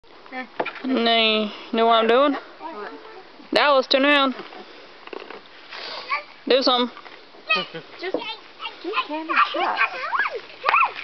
Kids first trip to Missouri
Dallas, Tristan and Wyatt's first lightbug catch